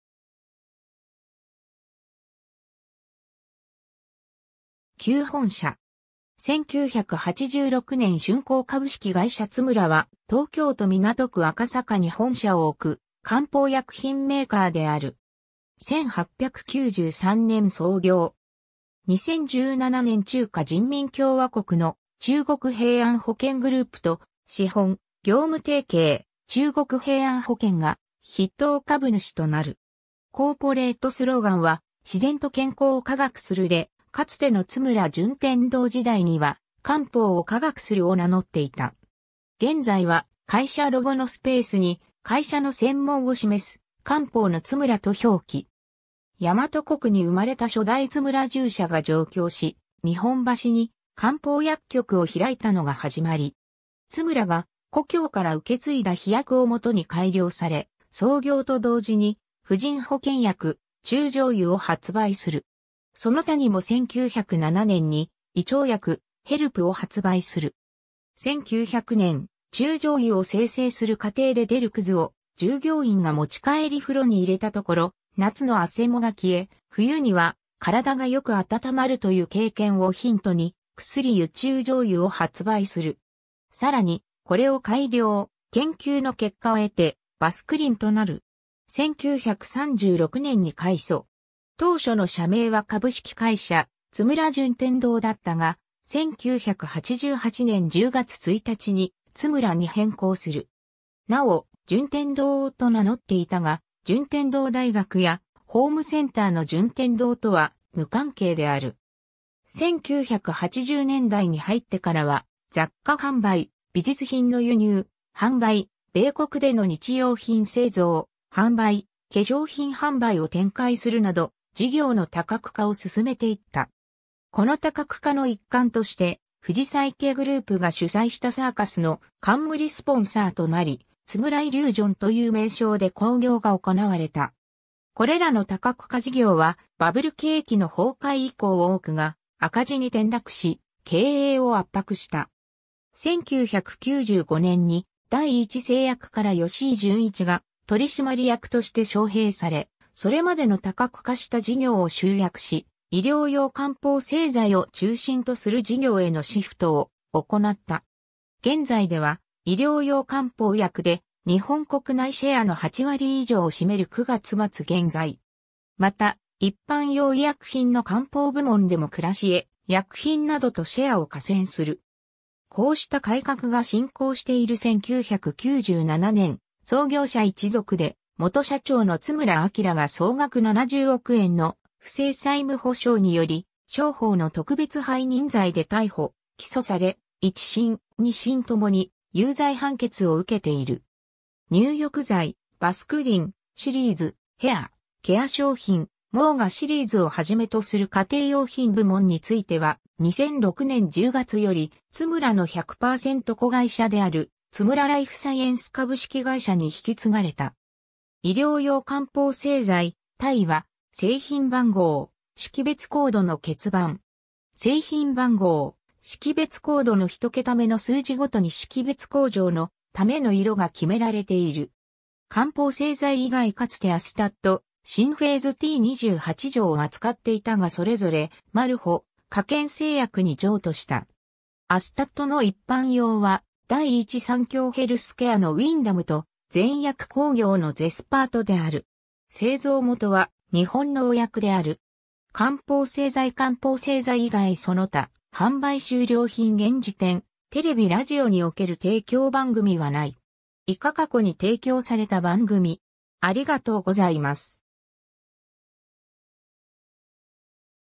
ツムラ
旧本社（千代田区二番町）。
1986年竣工 株式会社ツムラ（Tsumura & Co.）は、東京都港区赤坂に本社を置く漢方薬品メーカーである。
1893年創業。
2017年中華人民共和国の中国平安保険グループと資本・業務提携、中国平安保険が筆頭株主となる。
 コーポレート・スローガンは「自然と健康を科学する」で、かつての津村順天堂時代には「漢方を科学する」を名乗っていた。
現在は、会社ロゴのスペースに会社の専門を示す「漢方のツムラ」と表記。
 大和国（現在の奈良県）に生まれた初代津村重舎が上京し、日本橋に漢方薬局を開いたのが始まり。
津村が故郷から受け継いだ秘薬を元に改良され、創業と同時に、婦人保健薬「中将湯」を発売する。
その他にも1907年（明治40年）に胃腸薬「ヘルプ」を発売する。
1900年（明治33年）、中将湯を精製する過程で出るくずを従業員が持ち帰り風呂に入れたところ、夏のあせもが消え、冬には体がよく温まるという経験をヒントに、「くすり湯中将湯」を発売する。
さらにこれを改良・研究の結果を得て「バスクリン」となる。
 1936年（昭和11年）に改組。
当初の社名は株式会社 津村順天堂だったが、1988年（昭和63年）10月1日に「ツムラ」に変更する。
なお「順天堂」と名乗っていたが、順天堂大学やホームセンターのジュンテンドー（本社・島根県益田市）とは無関係である。
 1980年代に入ってからは、雑貨販売、美術品の輸入・販売、米国での日用品製造・販売、化粧品販売を展開するなど、事業の多角化を進めていった。
この多角化の一環としてフジサンケイグループが主催したサーカスの冠スポンサーとなり、「ツムライリュージョン」という名称で興行が行われた。
これらの多角化事業は、バブル景気の崩壊以降多くが赤字に転落し、経営を圧迫した。
 1995年（平成7年）に、第一製薬（現：第一三共）から芳井順一が取締役として招聘され、それまでの多角化した事業を集約し、医療用漢方製剤を中心とする事業へのシフトを行った。
現在では、医療用漢方薬で日本国内シェアの8割以上を占める（2007年（平成19年）9月末現在）。
また、一般用医薬品 (OTC) の漢方部門でもクラシエ薬品などとシェアを寡占する（業界3位）。
 こうした改革が進行している1997年（平成9年）、創業者一族で元社長の津村昭が総額70億円の不正債務保証により商法の特別背任罪で逮捕・起訴され、一審・二審ともに有罪判決を受けている。
 入浴剤「バスクリン」シリーズ、ヘア・ケア商品「モウガ」シリーズをはじめとする家庭用品部門については、2006年（平成18年）10月よりツムラの100%子会社であるツムラ ライフサイエンス株式会社（現在は、ツムラから完全独立後にアース製薬傘下に入り、株式会社バスクリンとなっている）に引き継がれた。
 医療用漢方製剤 「-」は製品番号/識別コードの欠番。
製品番号／識別コードの1桁目の数字ごとに識別向上のための色が決められている。
 漢方製剤以外 かつてアスタット、シンフェーズT28錠を扱っていたがそれぞれマルホ、科研製薬に譲渡した。
アスタットの一般用は第一三共ヘルスケアのウィンダムと全薬工業のゼスパートである。
製造元は日本農薬である。
 漢方製剤 漢方製剤以外 その他 販売終了品 現時点、テレビ・ラジオにおける提供番組はない。
 以下過去に提供された番組